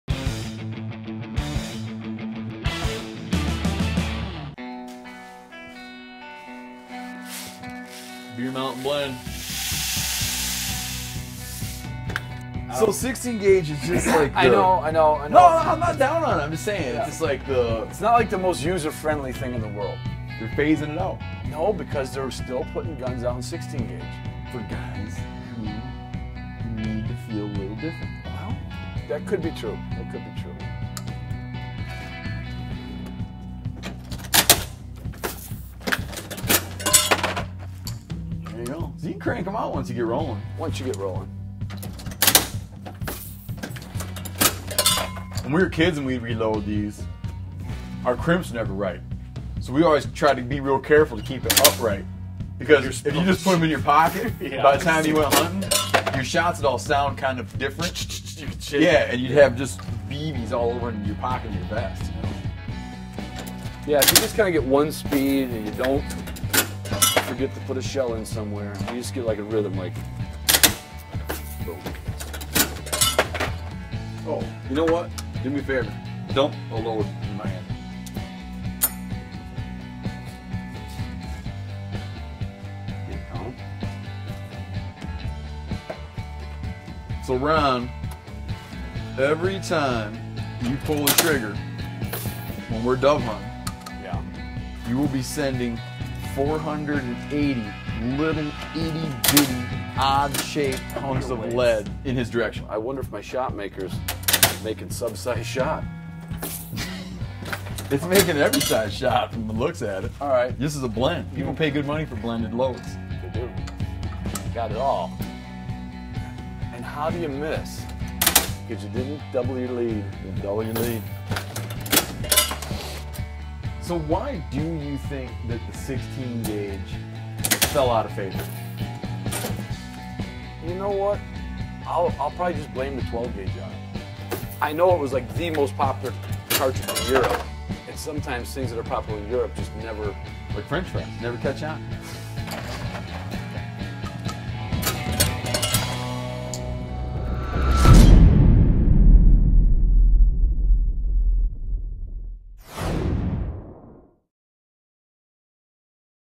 Steven Rinella Makes 16 Gauge Shells with Ron Boehme
Steven Rinella hangs with long-time friend Ron Boehme as Ron makes his own shells for his 16 gauge shotgun.